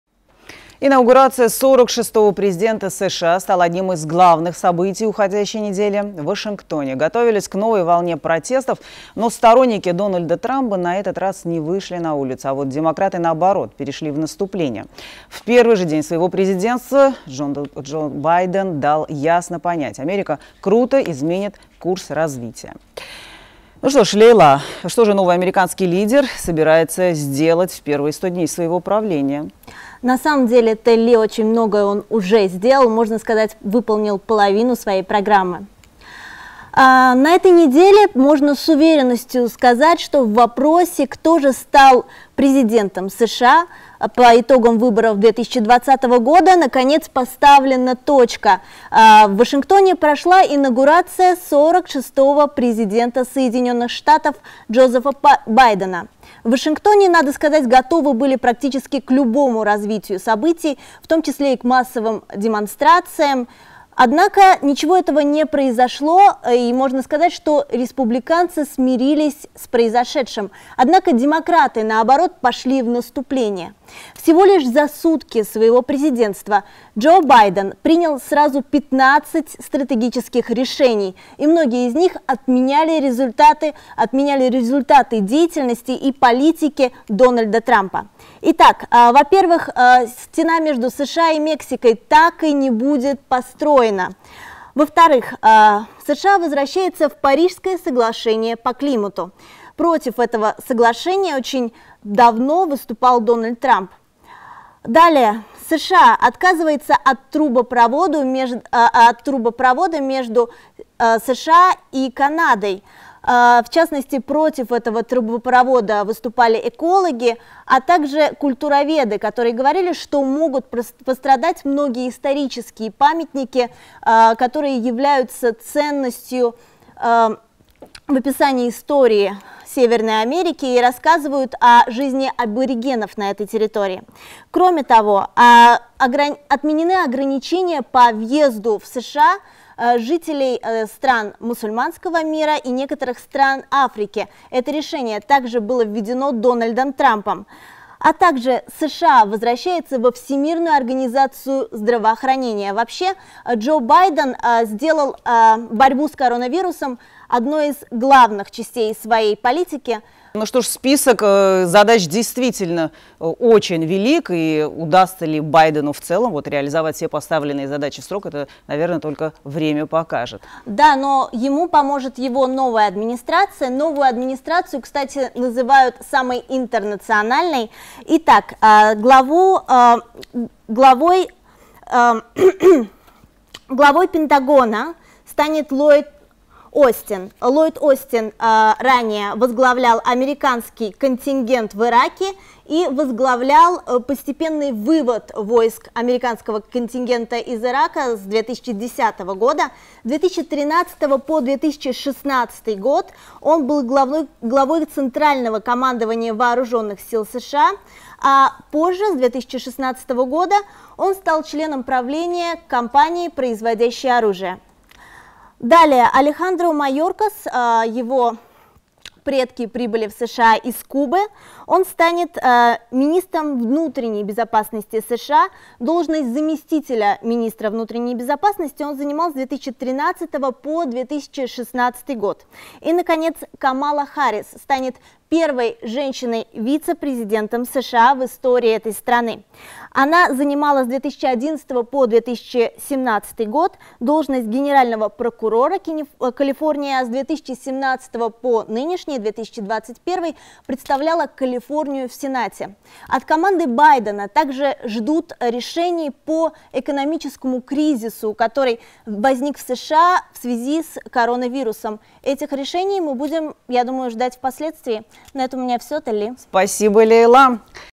Сторонники Трампа не вышли на улицы
Инаугурация 46-го президента США стала одним из главных событий уходящей недели.  В Вашингтоне готовились к новой волне протестов, но сторонники Дональда Трампа на этот раз не вышли на улицы. А демократы перешли в наступление. В первый же день своего президентства Джозеф Байден дал ясно понять - Америка круто изменит курс развития.